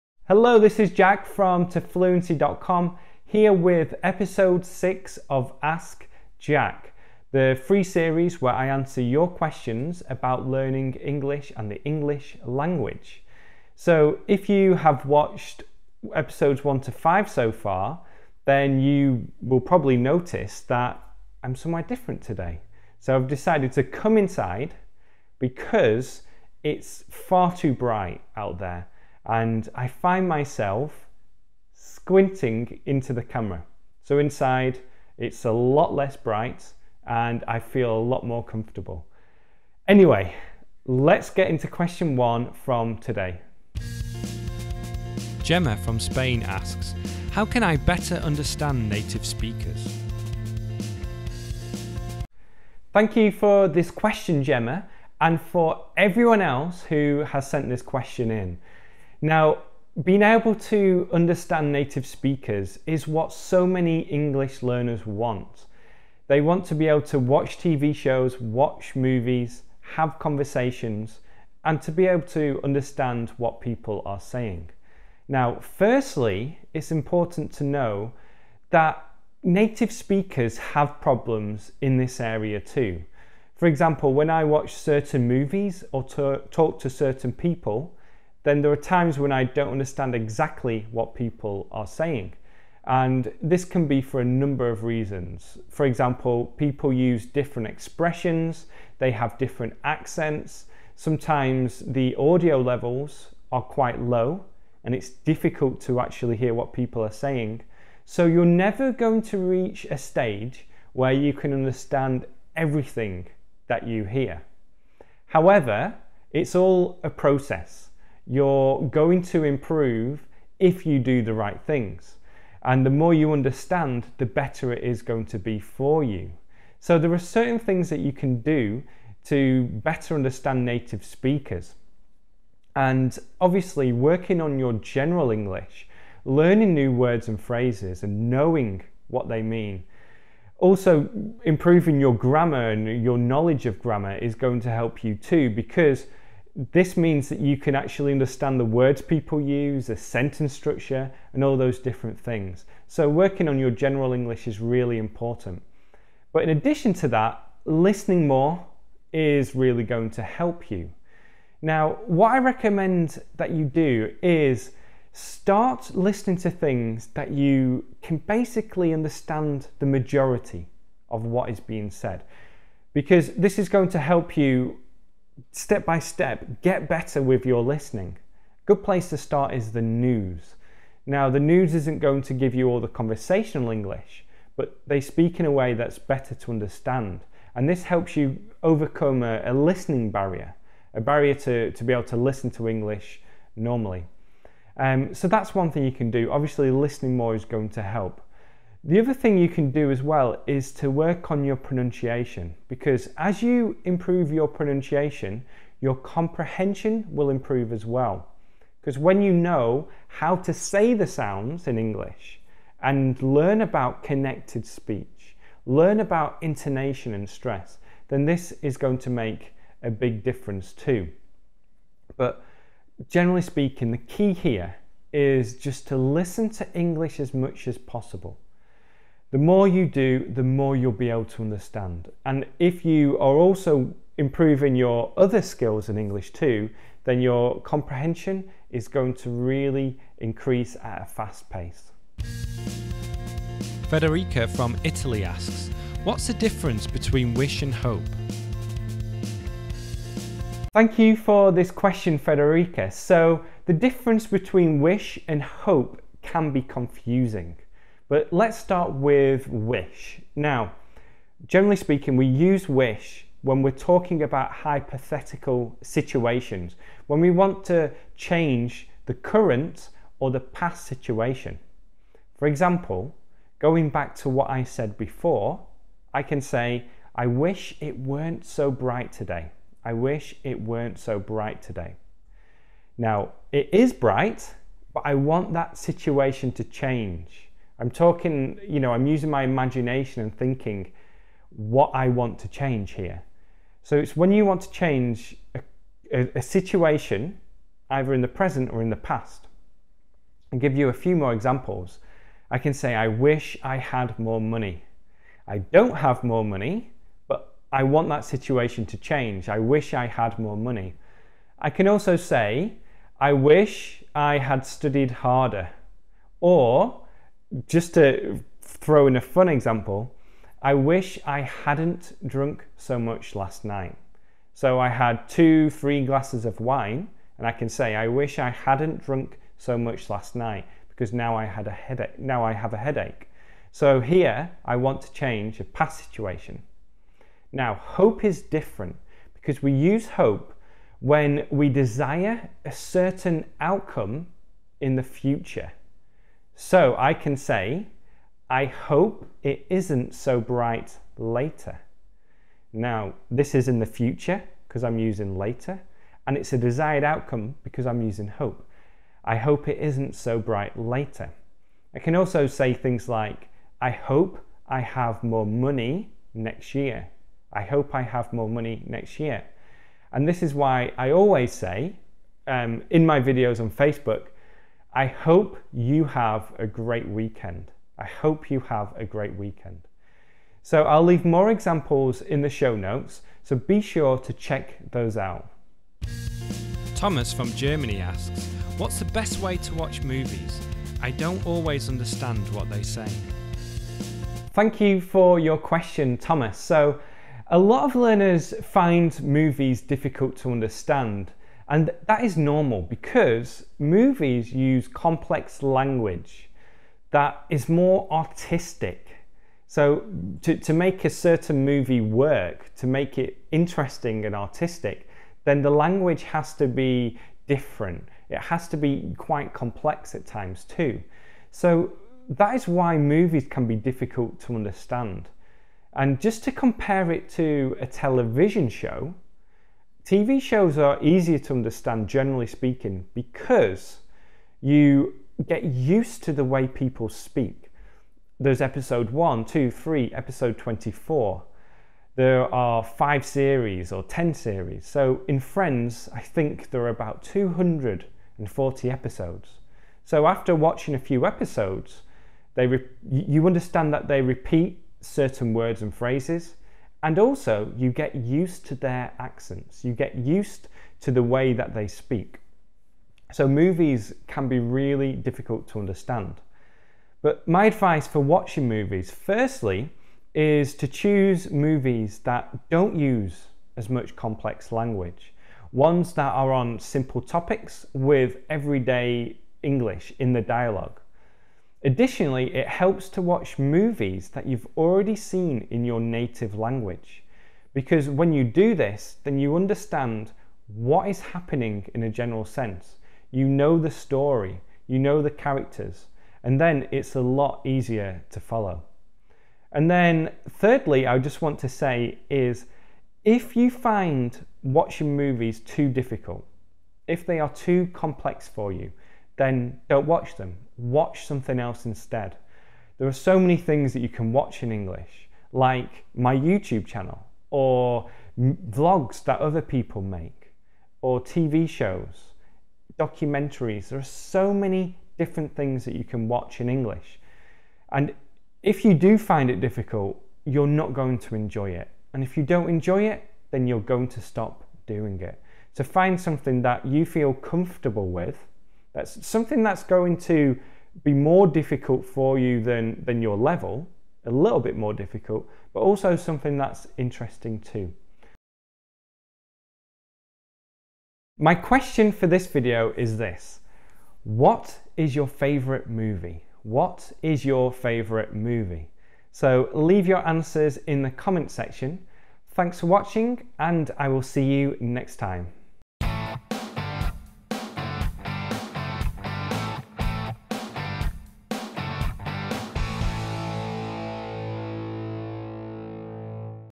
How to Understand Native English Speakers, Wish vs Hope, and Watching Movies (AJ #6)
How to Understand Native Speakers - In this first episode of "Ask Jack" I talk about which English accent to learn, how to know if you're making mistakes, and how to learn the different vowel sounds.

Q1: (0:43) - Understand Native Speakers
Q2: (4:02) - Wish vs Hope
Q3: (6:25) - Movies

SUMMARY

Being able to understand native speakers take time. There is a lot you need to do to reach this stage. Additionally, it's important to know that even native speakers have problems understanding other native speakers at times. In addition to improving your overall English, I mentioned two things in this video: improving your pronunciation will help you better understand native speakers and you can slow down real speech to help you understand what people are saying, and then speed this back up to help you get used to the speed.

Wish is used for hypothetical situations where you want to change. For example, in the video I said, "I wish it wasn't so bring today." It was bright, but I wanted to change that. I also said, "I hope it isn't so bring later." So, we use hope when we are talking about a desired outcome in the future.

Movies are difficult to understand because of the langauge that they use. Therefore, I recommend watching movies that you've already seen in your native language and movies that are easier to understand. If you don't enjoy watching movies, then watch something else instead, like TV shows.